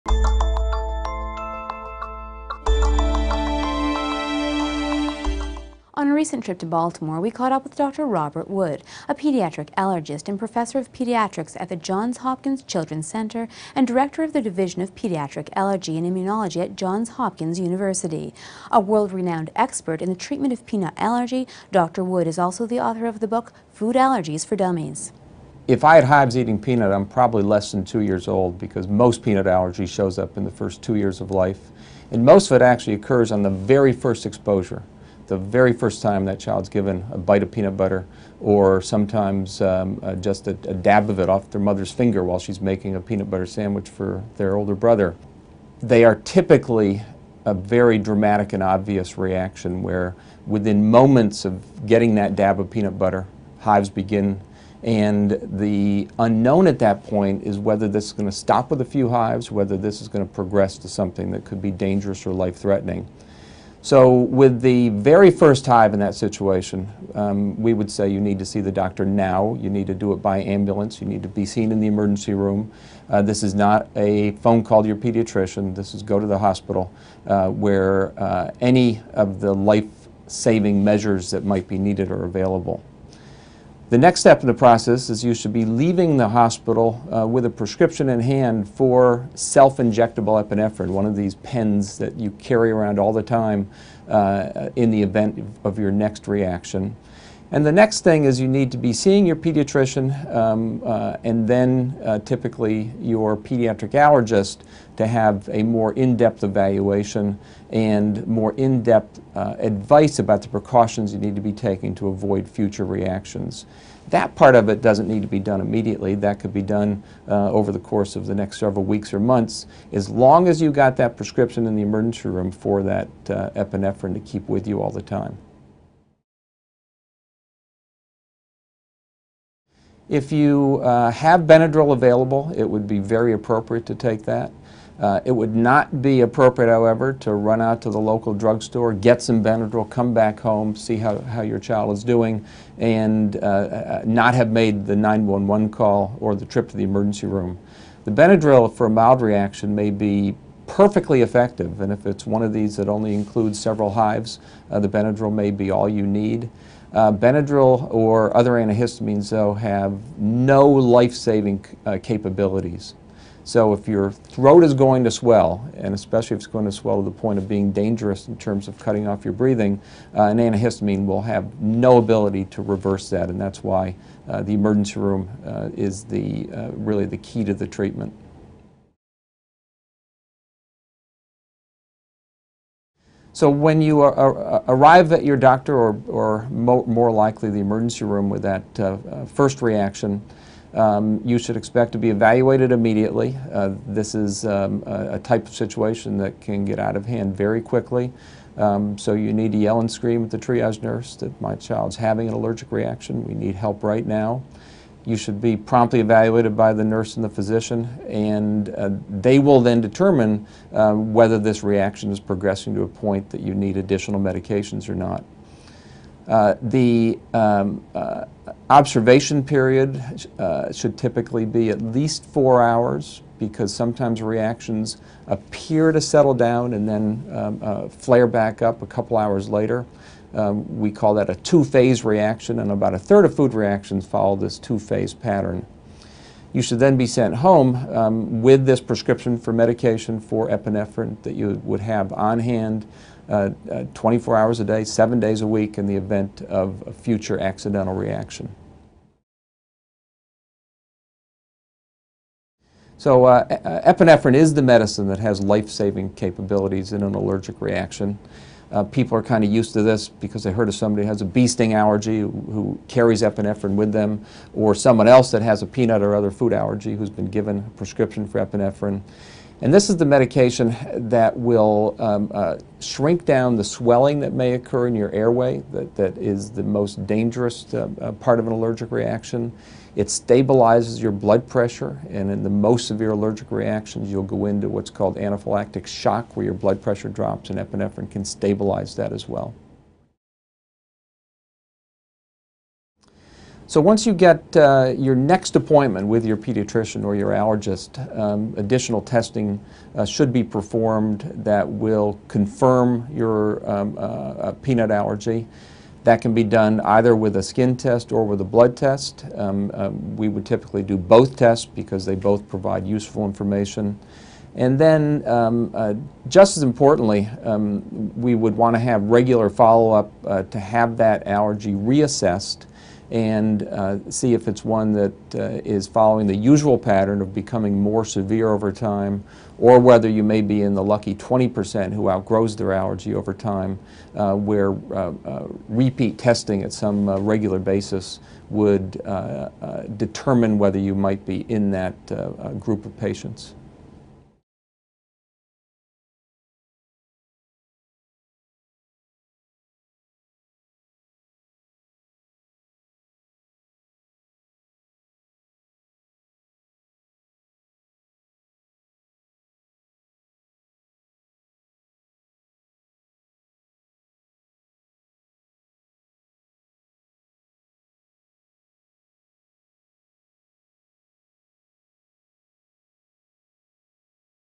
Allergic Reactions - Dr Robert Wood, MD
Dr. Robert Wood, MD, pediatric allergist and professor of pediatrics at the Johns Hopkins Children's Center, and director of the Division of Pediatric Allergy and Immunology at Johns Hopkins University.